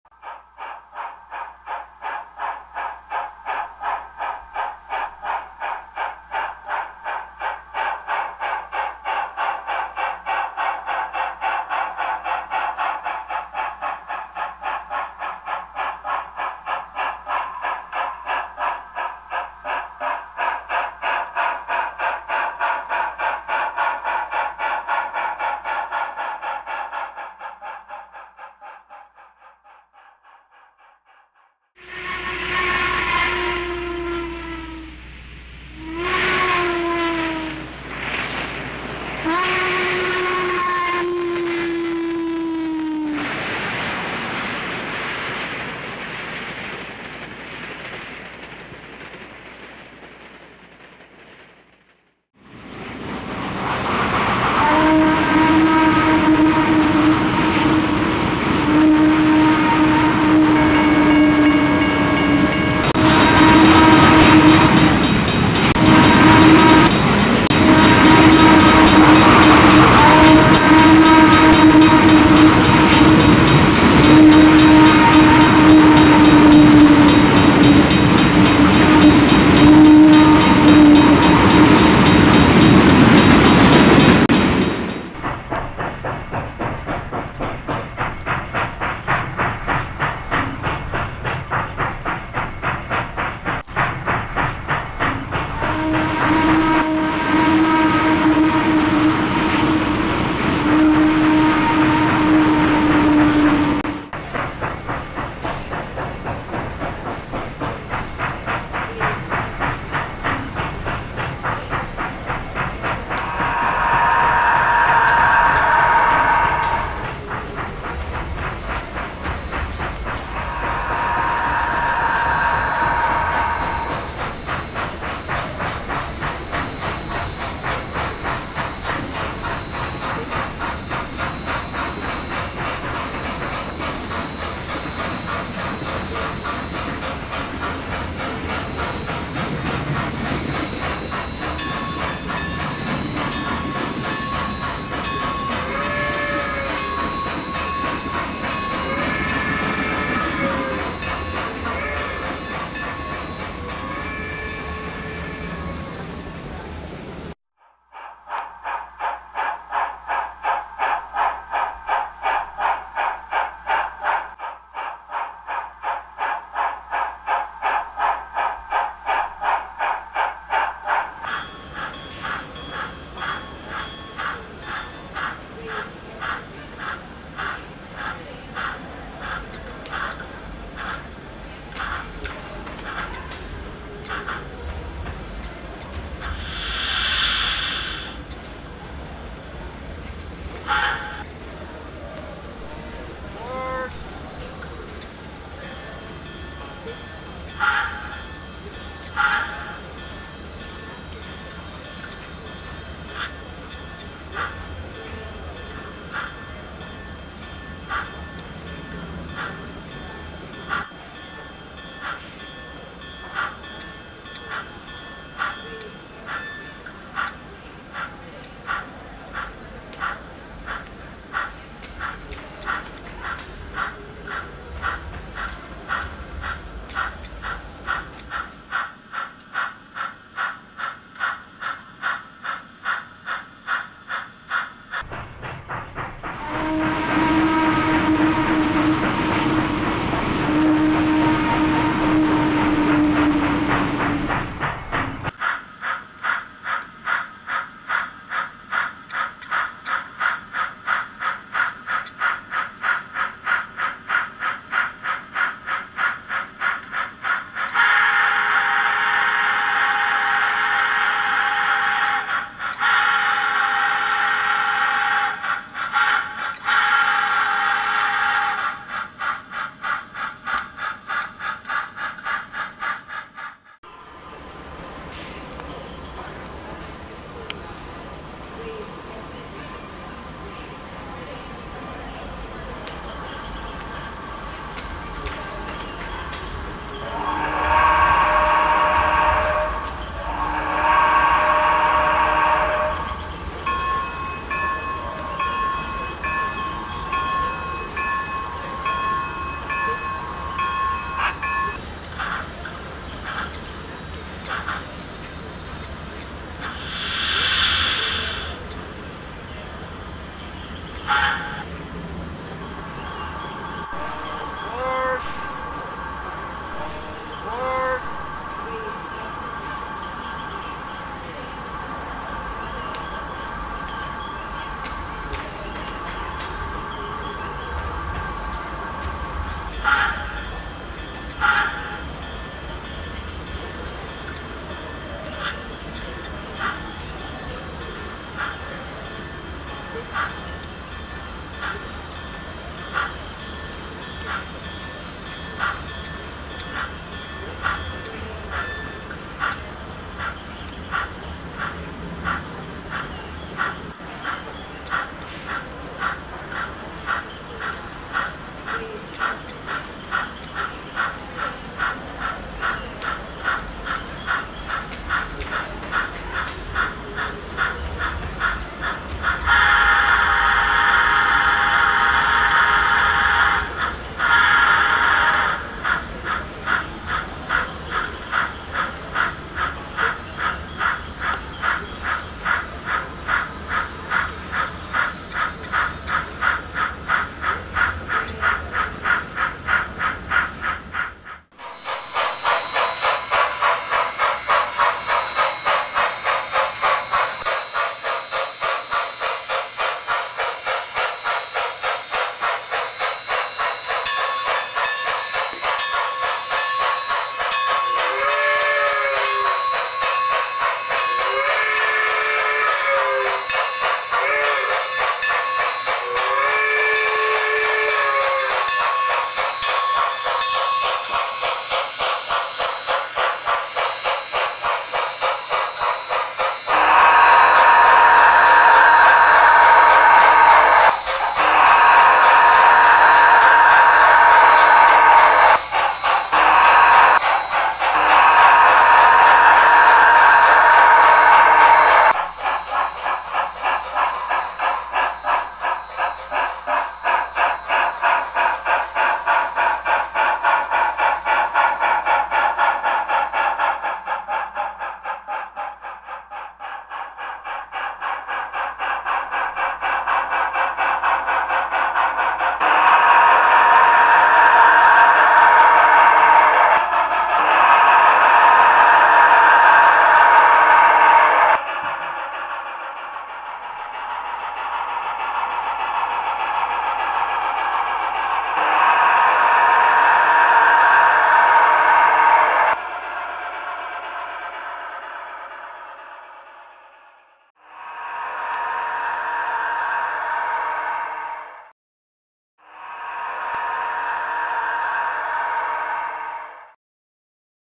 CHASING THE EM 1 (HD)
This video, created on my SV&GS layout, focuses on the new N scale 2-8-8-4 EM-1 steam locomotive by Bachmann. It's DC/DCC equipped and is seen here running in DC mode. I've augmented the video by adding audio clips. Soundtraxx Tsunami sound of the HO version of the EM-1 and actual prototype sound clips were used for the audio tracks.